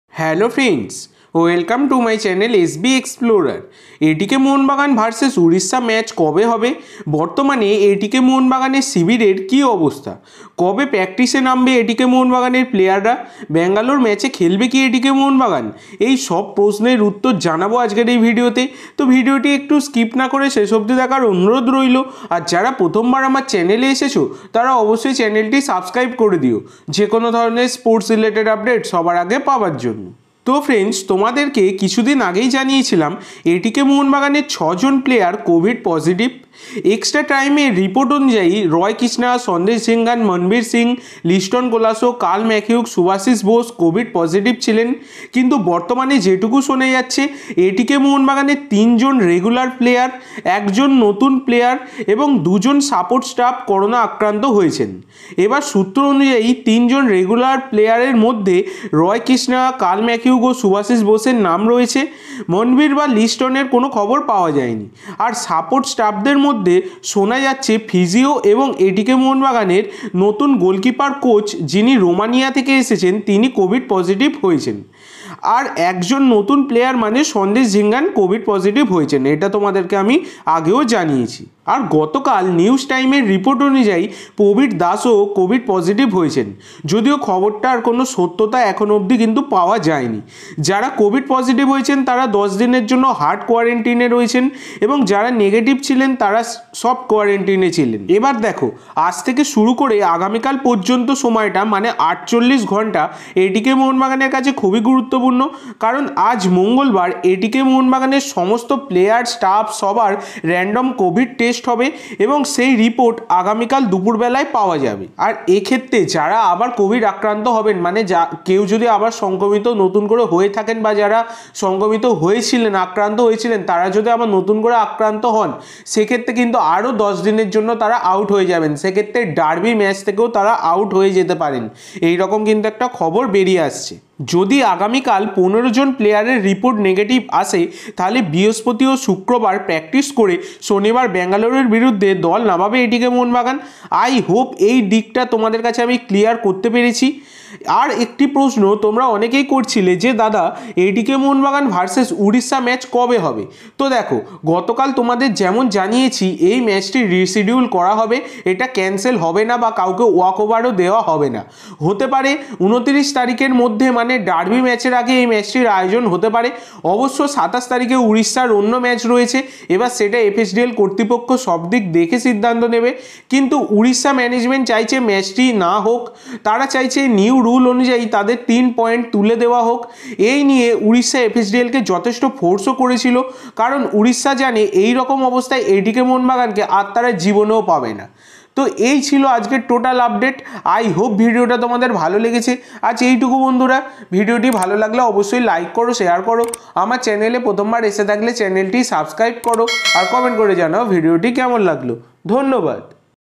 ATKMB 6 Players Covid Positive😷ATKMB VS Odisha ম্যাচ কবে হবে?কবে প্র্যাকটিস করবে প্লেয়াররা?🔥
Comment ⌨️ করুন।

ধন্যবাদ🙏🙏🙏🙏🙏